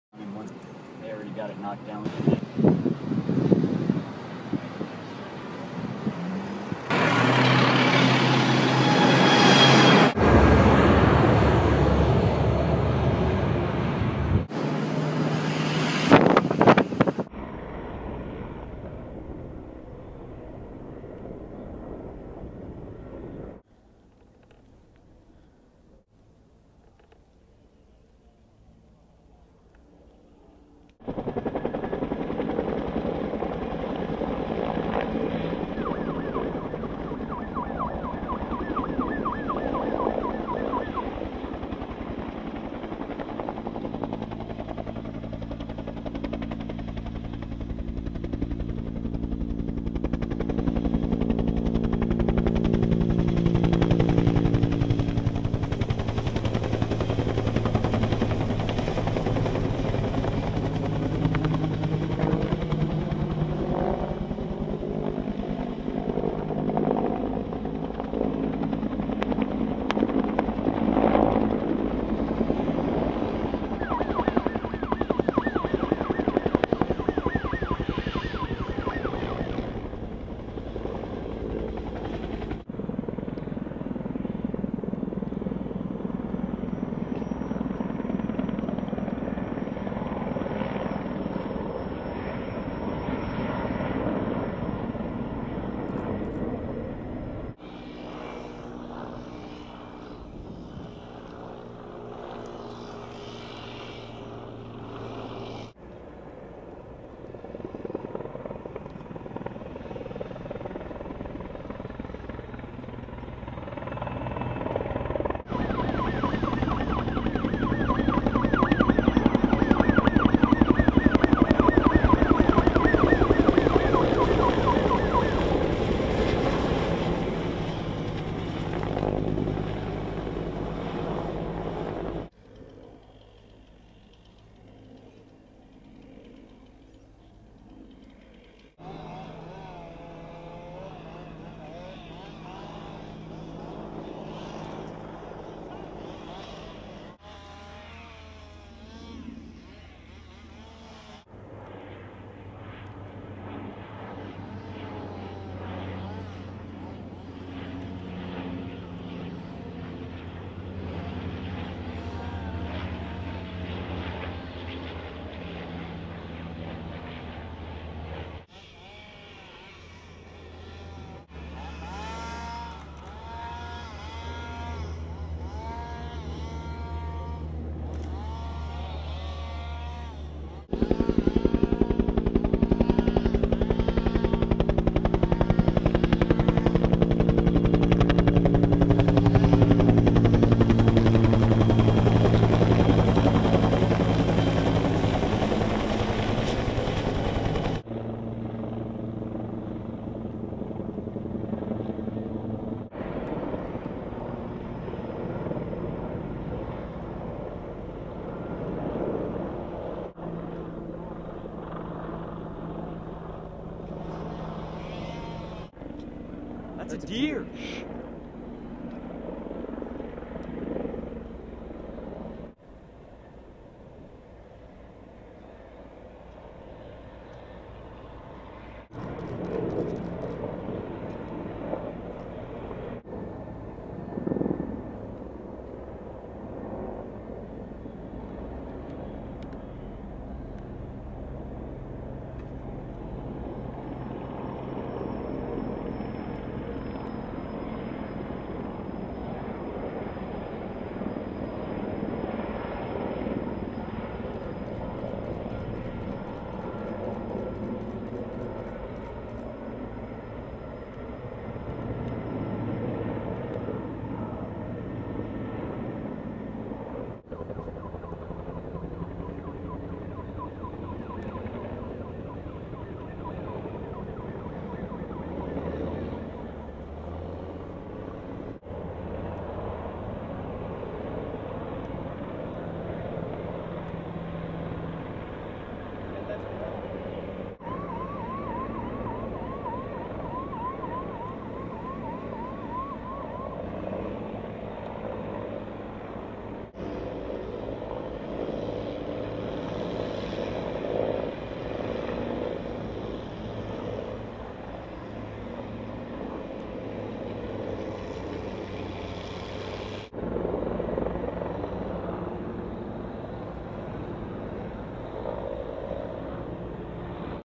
VW Bug catches fire ignites mountains ablaze 8/18/2013
1967 Volkswagen VW Bug catches fire and ignites a blaze in the mountains of Calabasas, next to Malibu Ca. Several homes were in danger. No damage was reported.